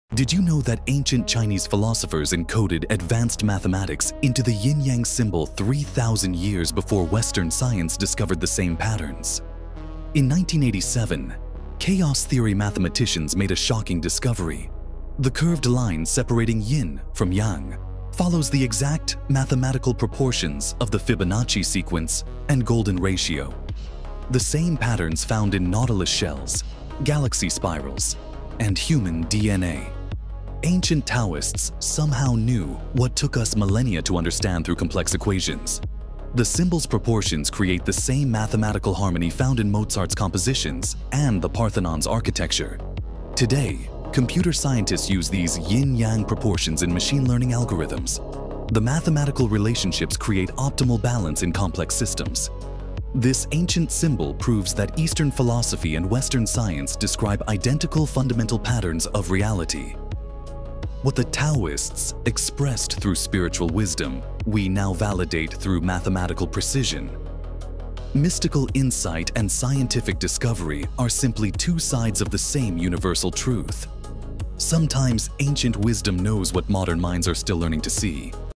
Ancient Yin Yang Symbol Hides the Fibonacci Sequence Inside
Mind blown 🤯 Ancient wisdom meets modern math